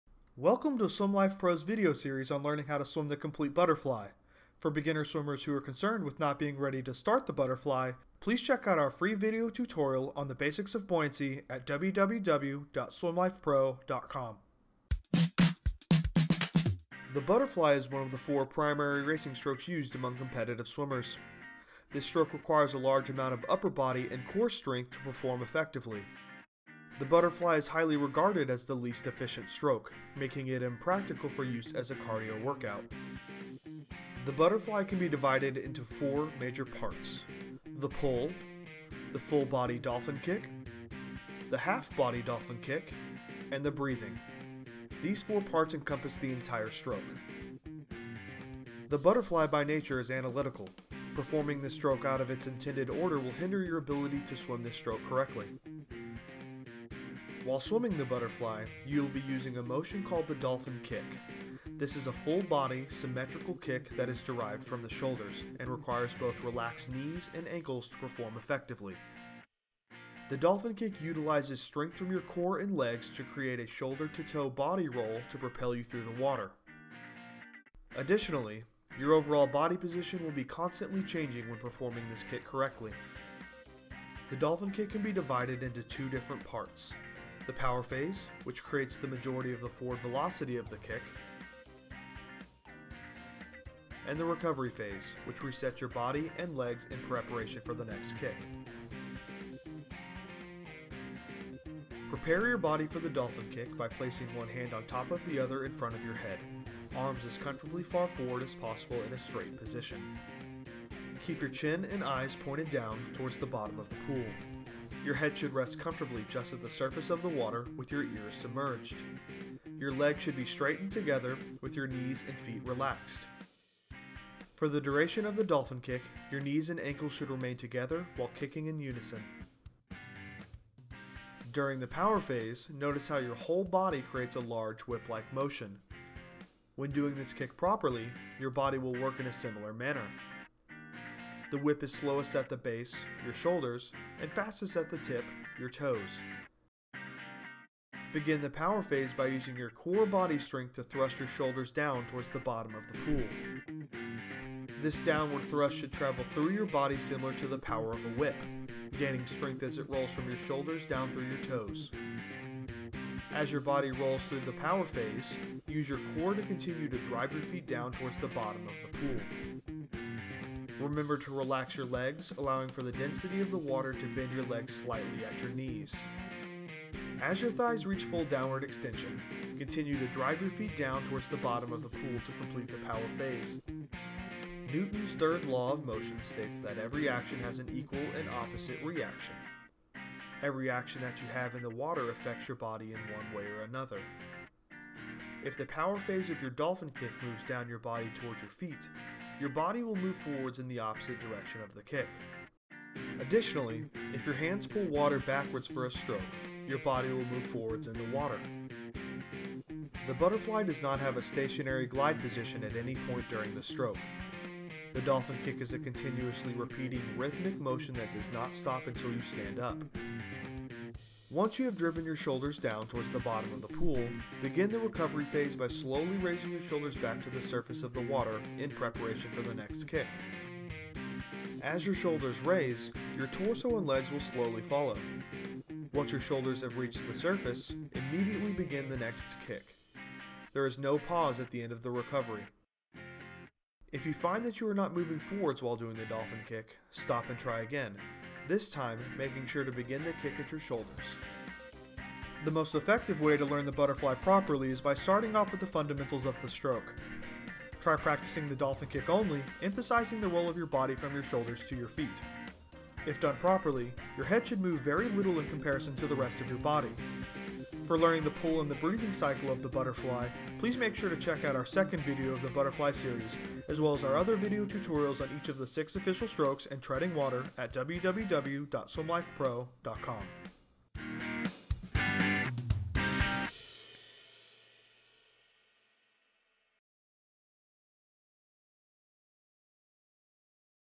Learn to Swim - Butterfly Part 1
SwimLifePro's Butterfly video tutorials review all aspects of what is required in order to learn how to properly swim the Butterfly both effectively and efficiently in the water. These skills will be the foundations of everything that you will need to accomplish in your journey to becoming a lifelong swimmer!